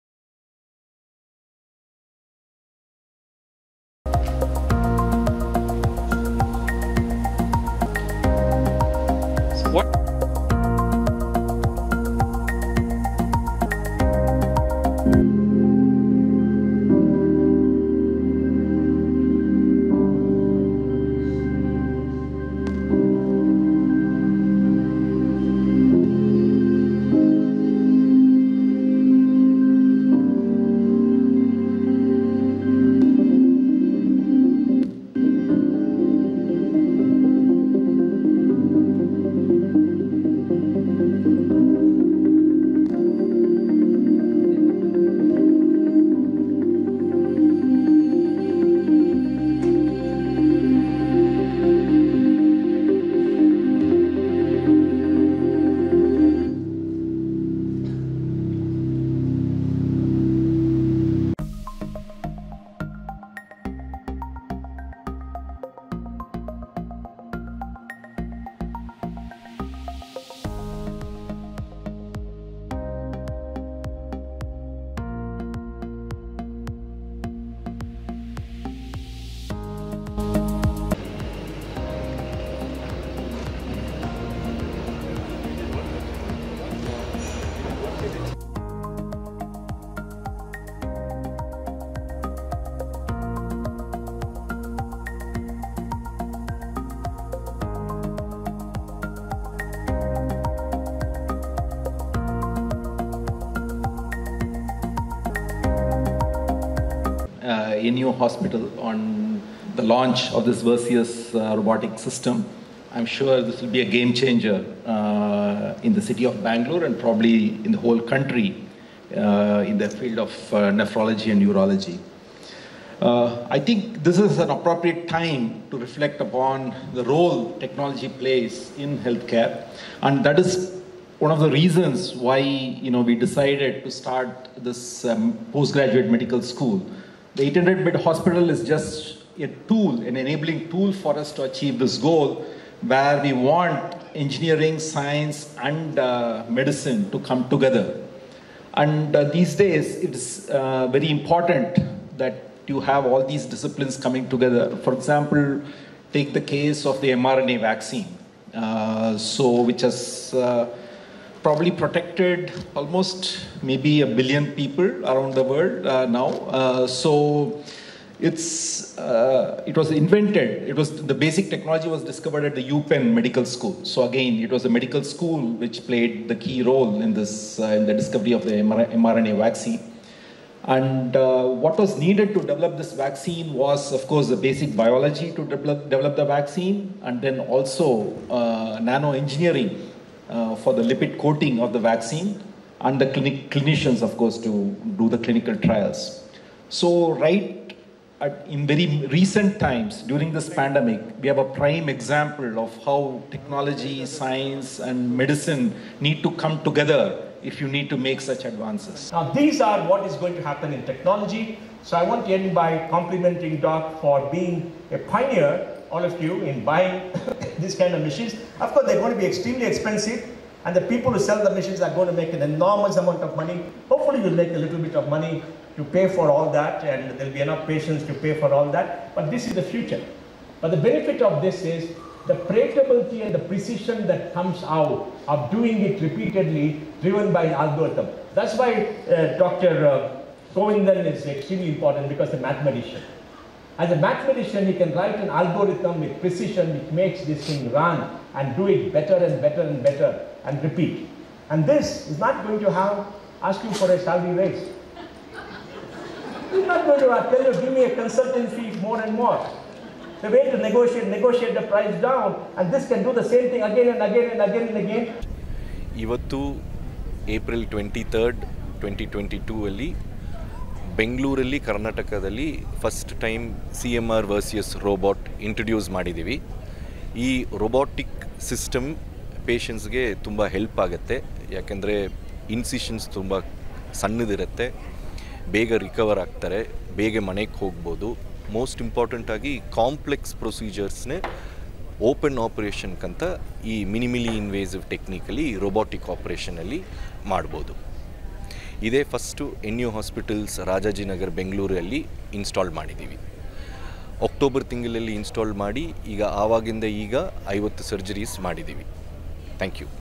NU Hospitals became the first in the country to introduce "Version Robotic Surgical Systerm"
NU Hospitals became the first Nephro Urology, Fertility super speciality Hospital in the country to introduce "Version Robotic Surgical Systerm"